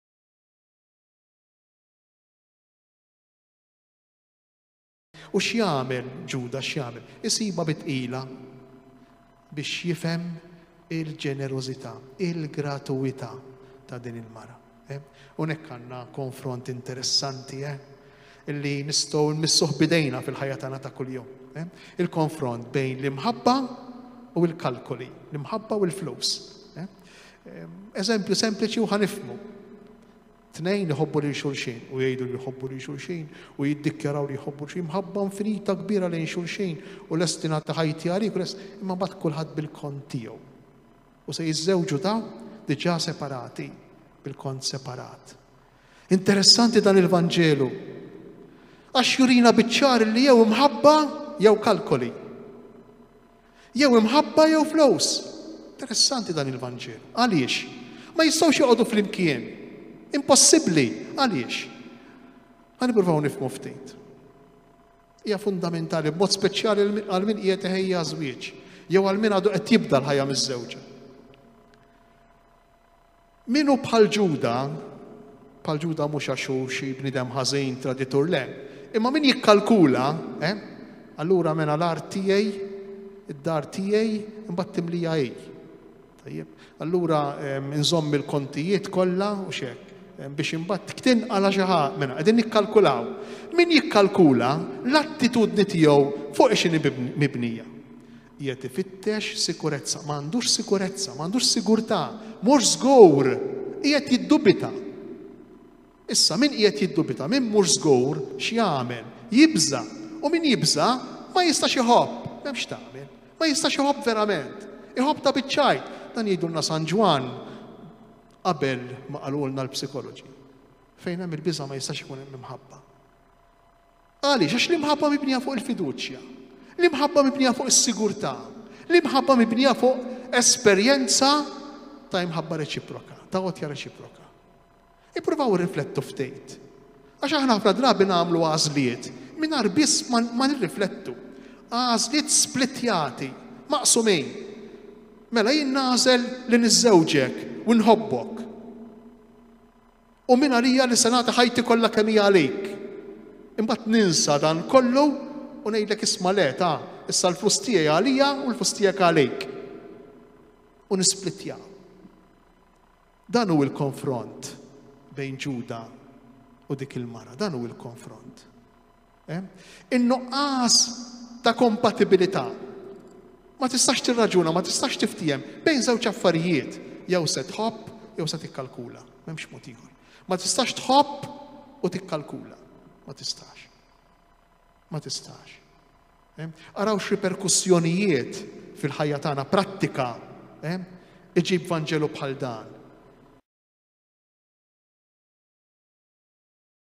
L-Isqof Anton Teuma. Mill-Lectio Divina tat-Tnejn 29/03/2021 (3)
Ġuda jsibha bi tqila biex jifhem il-ġenerożità, il-gratwità tal-imħabba ta’ Marija li tidlek riġlejn Ġesù bil-fwieħ. Mill-Lectio Divina. 29/03/2021.